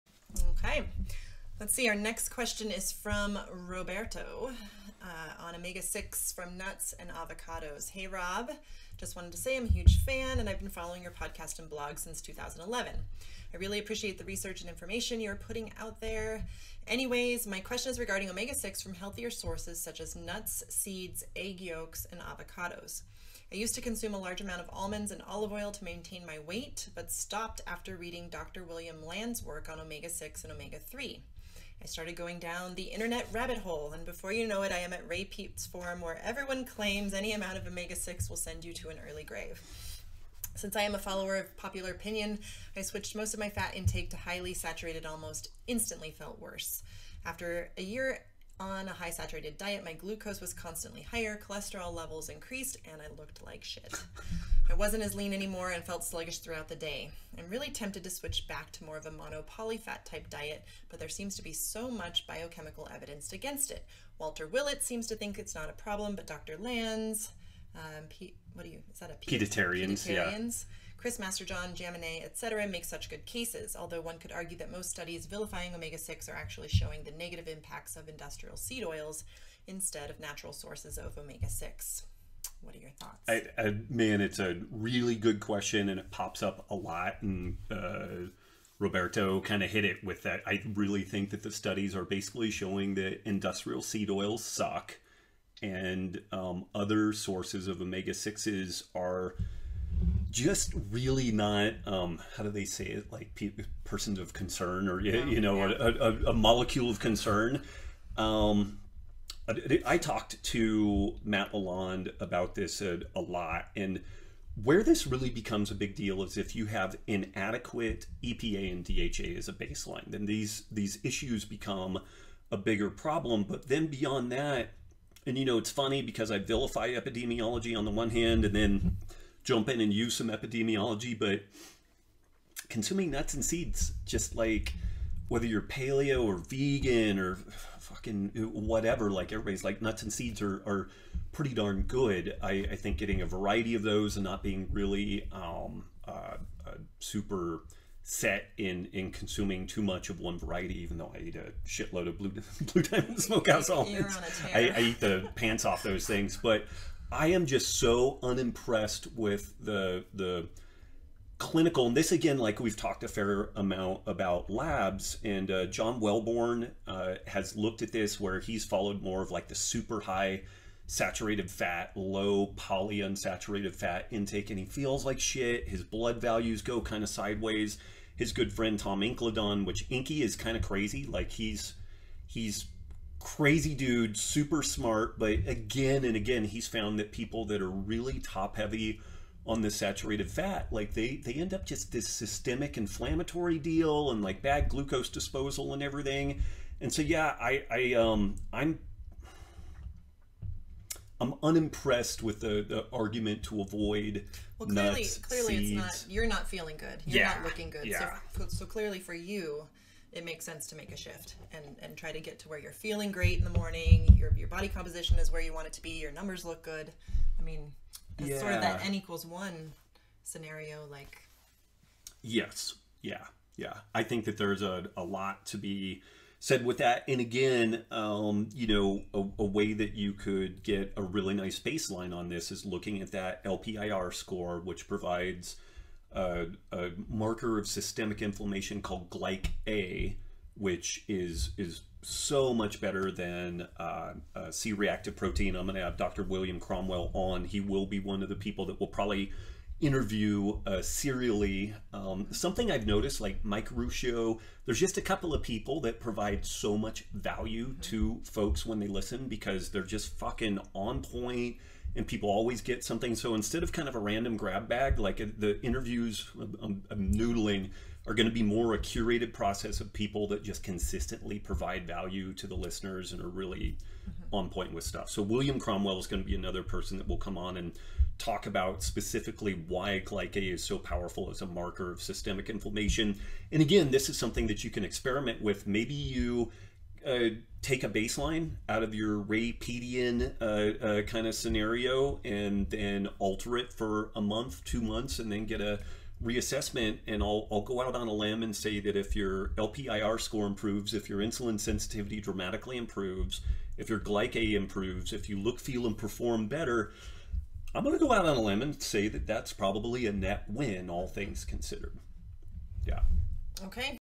Is Omega-6 from Nuts & Avocados OK? (Ep420 QA13 Q5)
5. [19:44] Omega-6 from nuts and avocados: Is Omega-6 from Nuts & Avocados OK?
Clip: 1:34 - 2:34

Roberto says:
Hey Robb, just wanted to say that I am a huge fan of your work, and I have been following your podcast and blog since 2011. I really appreciate the research and information you are putting out there. Anyways my question is regarding Omega 6 from healthier sources such as nuts, seeds, egg yolks and avocados. I use to consume a large amount of almonds and Olive oil to maintain my weight but stopped after reading Dr. William Lands work on Omega 6 and Omega 3. I started going down the internet rabbit hole and before you know it I am at a Ray Peat forum where everyone claims any amount of Omega 6 will send you to an early grave! Since I am a follower of popular opinion, I switch most of my fat intake to highly saturated and almost instantly felt worse. After about a year on a high saturated fat diet my glucose was constantly higher, cholesterol levels increased and I looked like shit. I wasn't as lean anymore and felt sluggish throughout the day.. I am really tempted to switch back to more of a mono/poly fat type diet, but there seems to be so much biochemical evidence against it. Walter Willet, seems to think it is not a problem, but Dr. Lands, Peatarians, Chris Masterjohn, Jaminet, etc., make such good cases. Although one could argue that most studies vilifying Omega 6 are actually showing the negative impacts of industrial seed oils instead of natural sources of Omega 6. Thoughts?